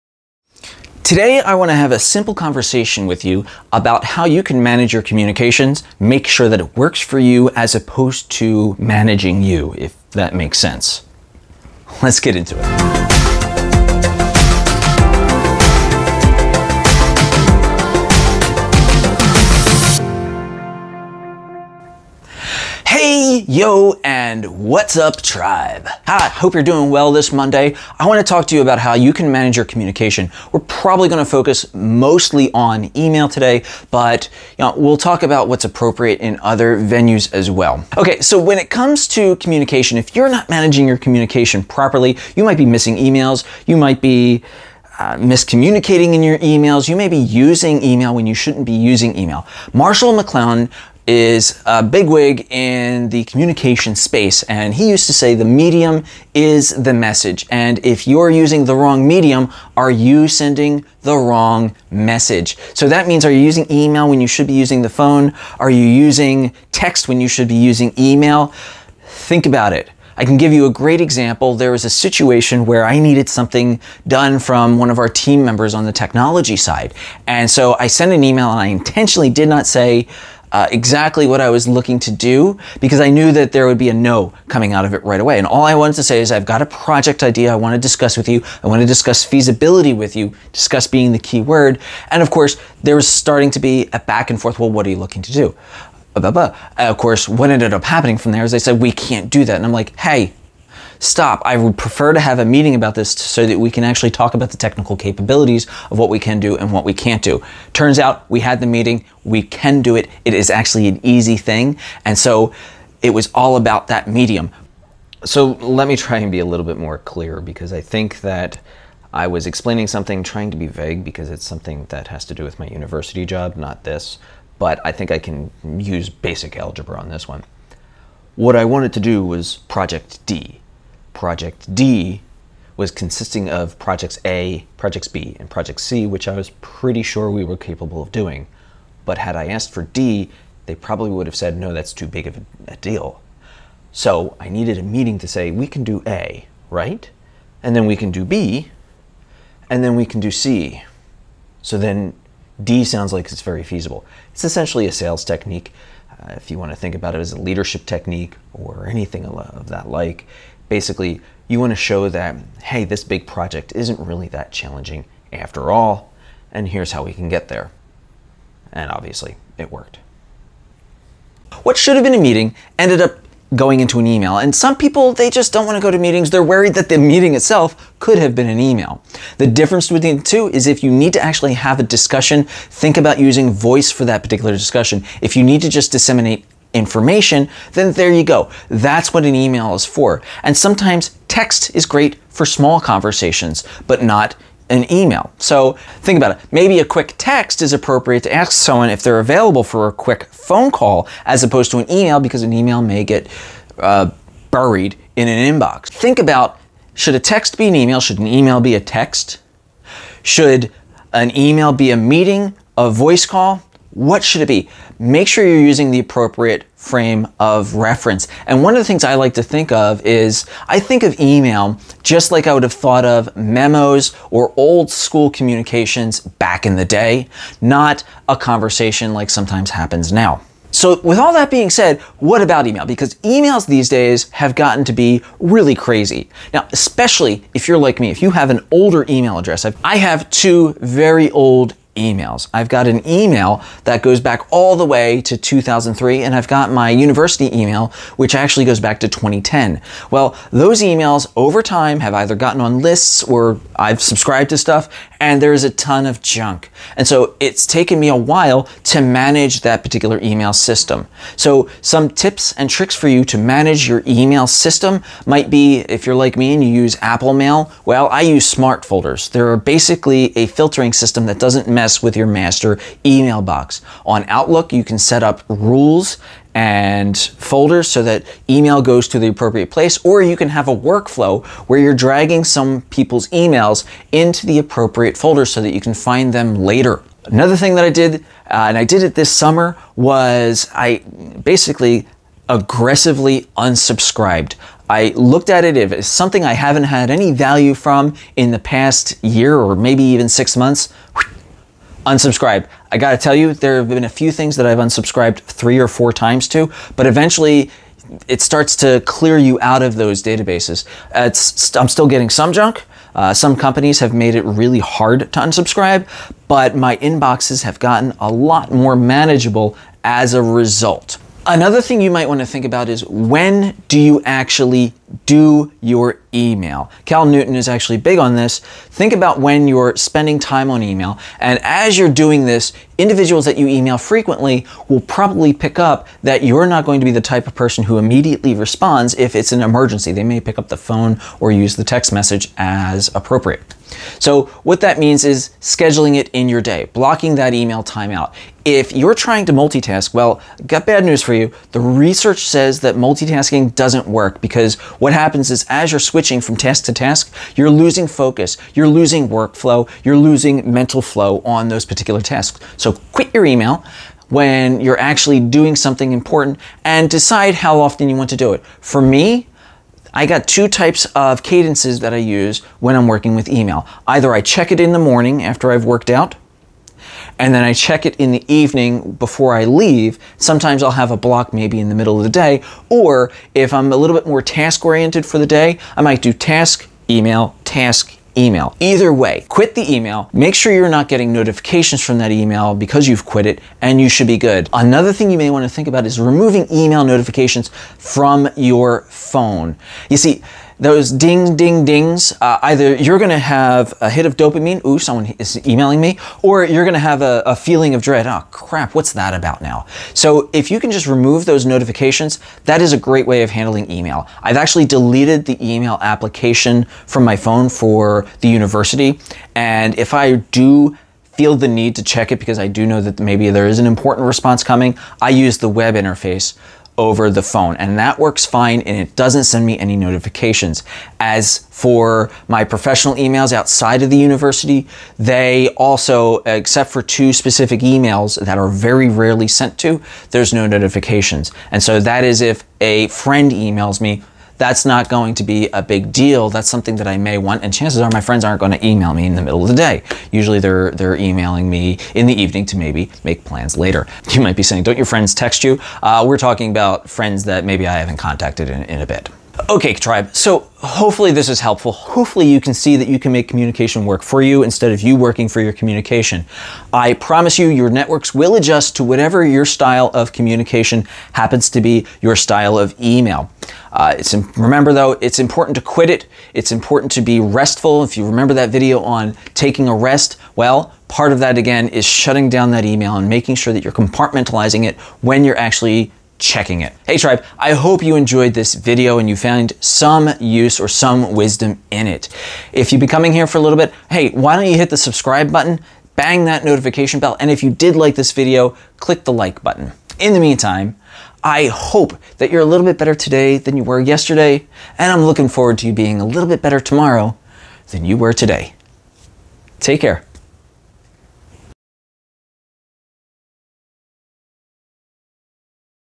2-4 Managing Communication
Does email manage you, or are you managing your email. Let's talk about some ways you can get ahold of this modern beast.